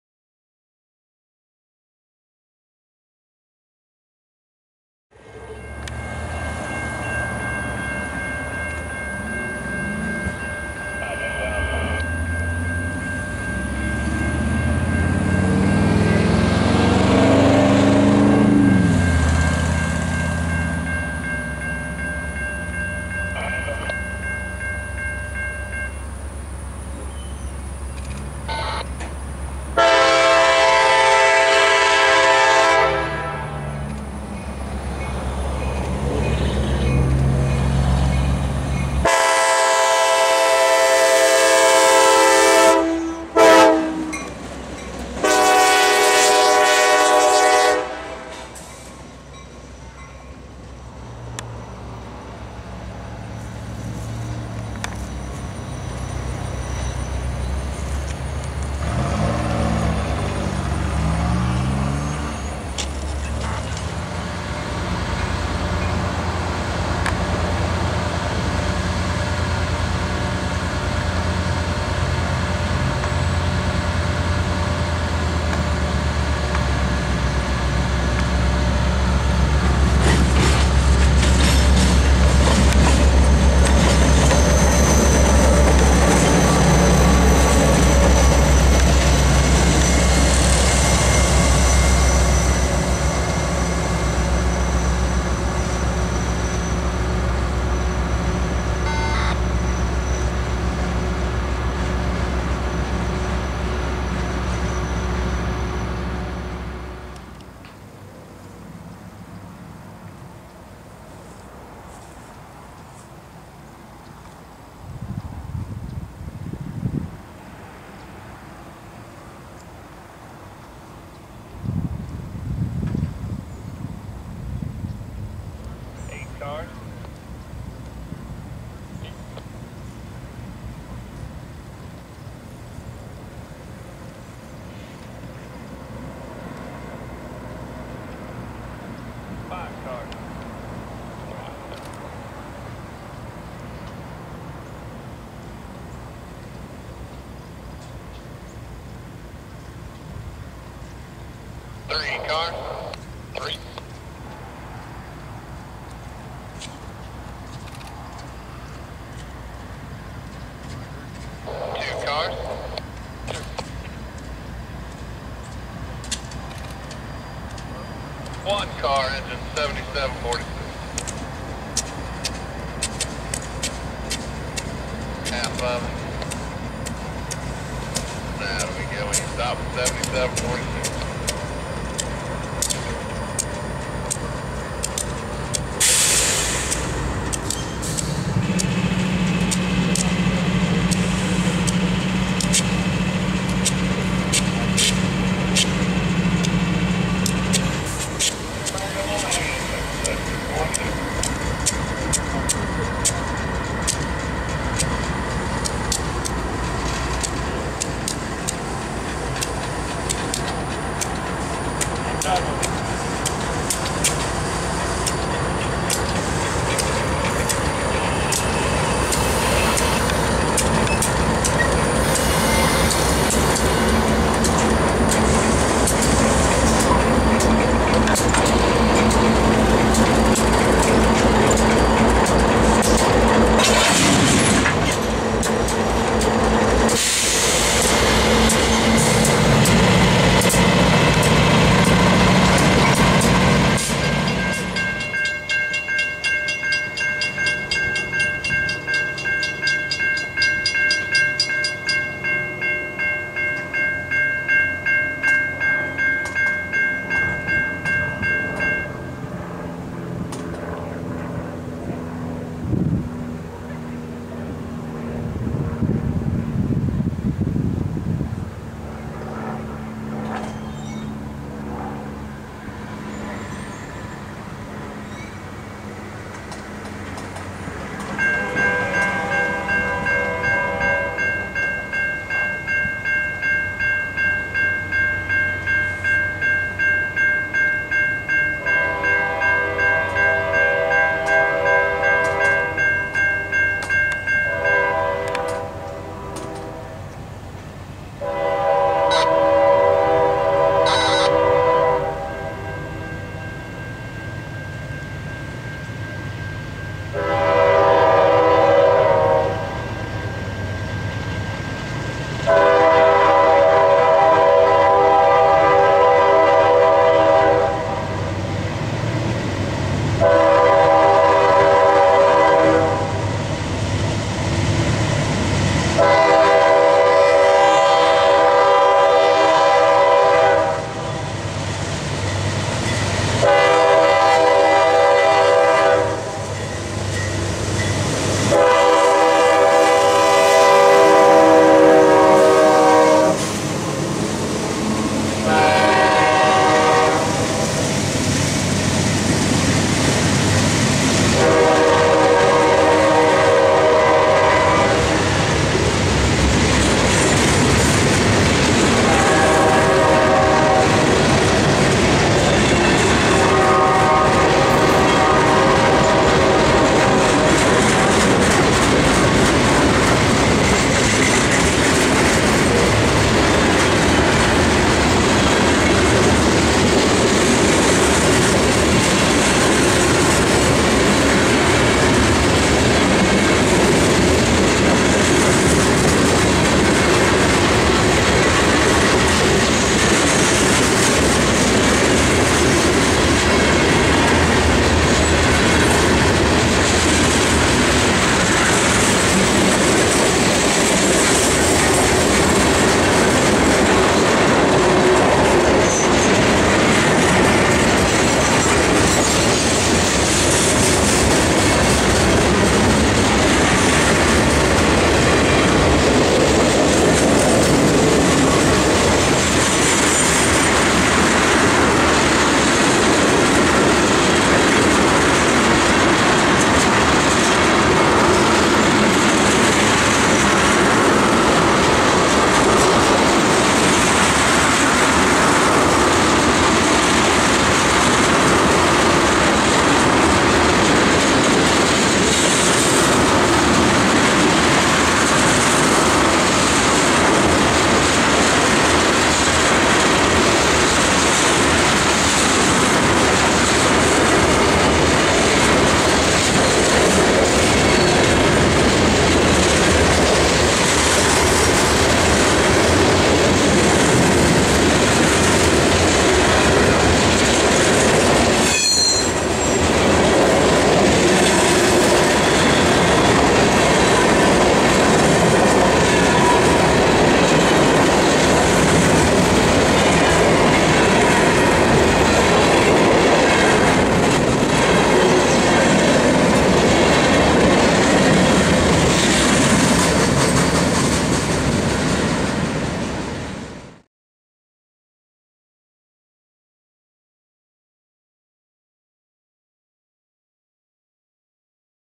CSX Q453's trip through Lakeland an AMAZING K5LA!!!
CSX Q453:
Engines:
CSX C40-8W #7746
CSX C40-8 #7582
CSX GP39-2 #4315
CSX GP39-2 #4317-beautiful K5LA!!!
Taken on 11-13-10 by Joe Robinson.